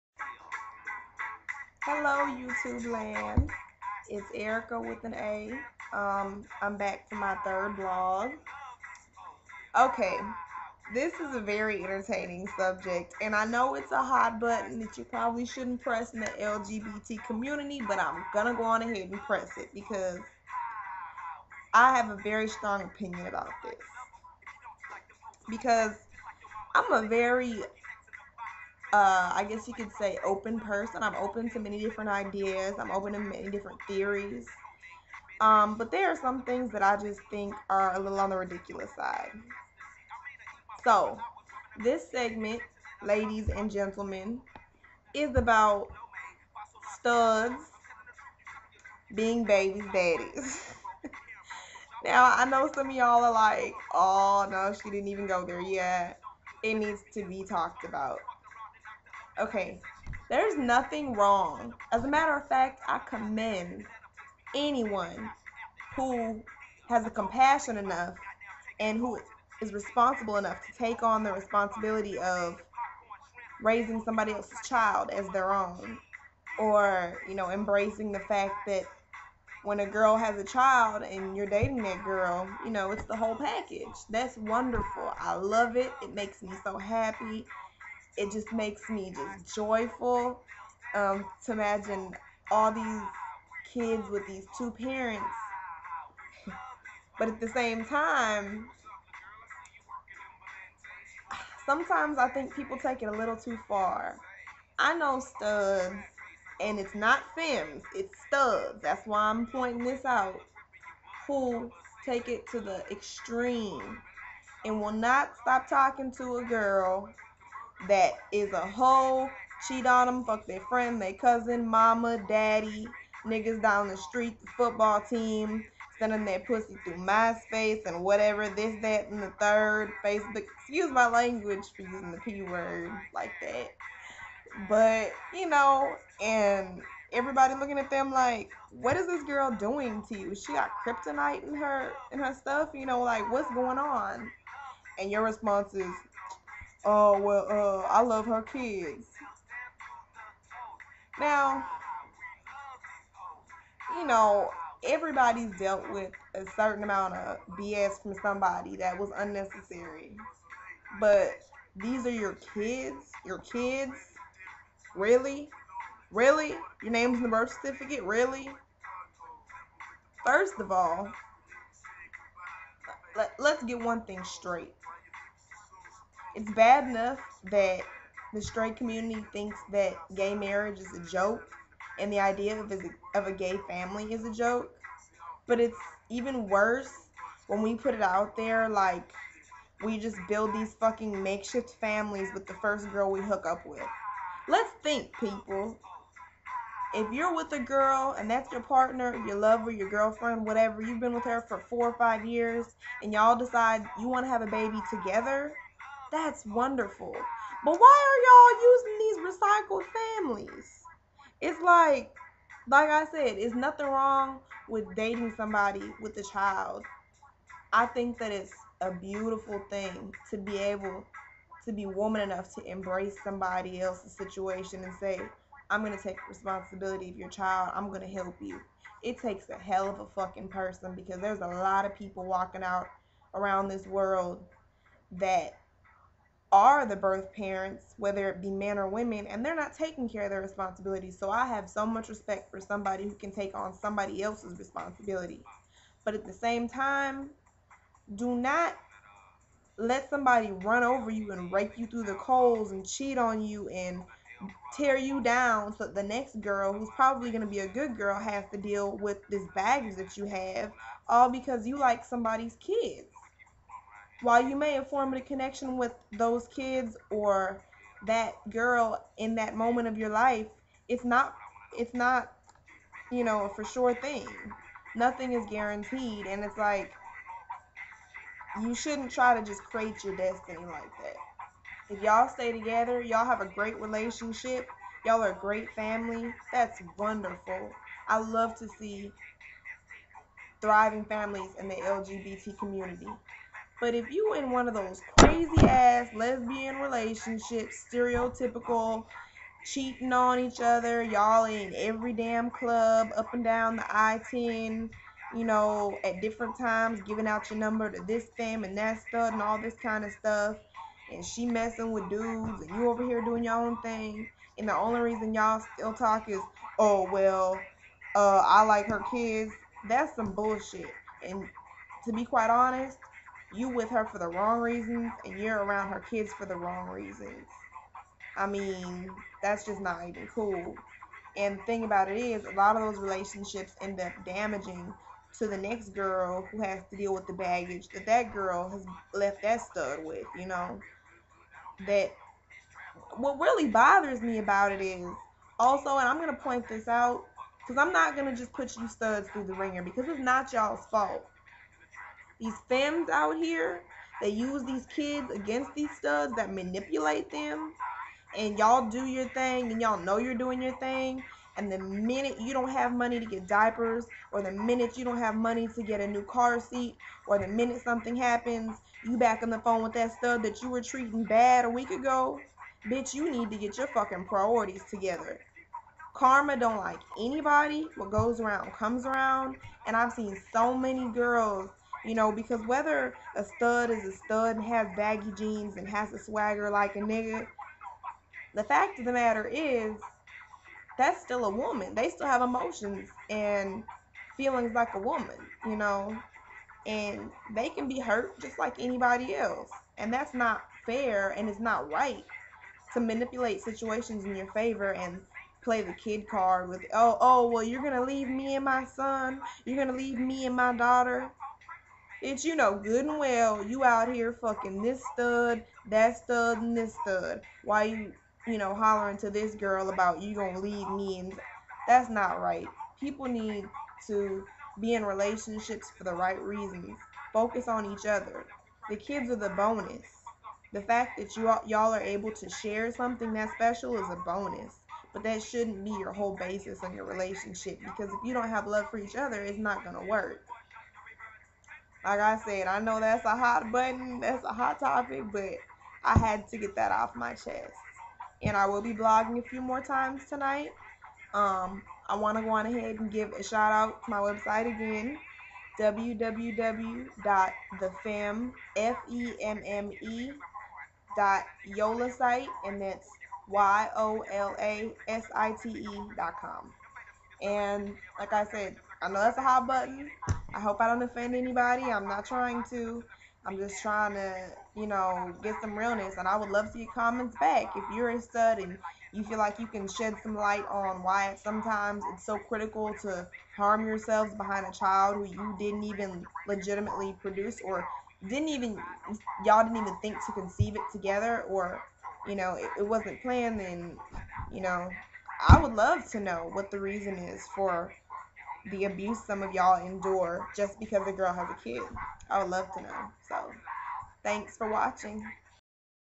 Stud "babys daddys" Confessions of a Femme #3
A question to the LGBT communtiy about why Studs stay with Femmes who treat them bad just because they are the "babys daddy".